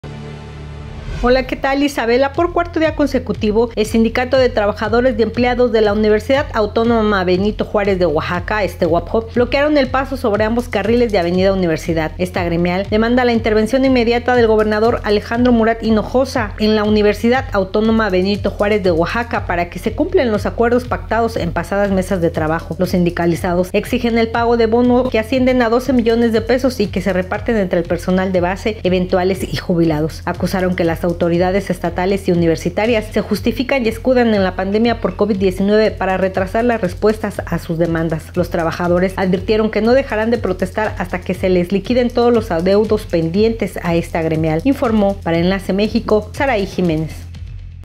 TRABAJADORES DE LA UABJO PIDEN PAGOS PENDIENTES AL GOBIERNO ESTATAL
Los trabajadores de la UABJO bloquean importante vialidad para exigir el pago pendiente por parte del estado de prestaciones.